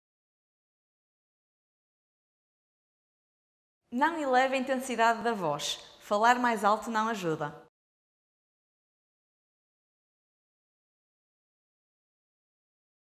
Terapêutica da Fala UFP | Consciencialização para a Afasia | 23.ª dica
23.ª dica  - Não eleve a intensidade da voz. Falar mais alto não ajuda.

O mês de junho fica marcado pela campanha internacional de consciencialização para a afasia, uma perturbação de linguagem e comunicação que surge na sequência de uma lesão neurológica.  Durante todo o mês, a licenciatura em Terapêutica da Fala UFP vai partilhar estratégias de comunicação para lidar com a Afasia.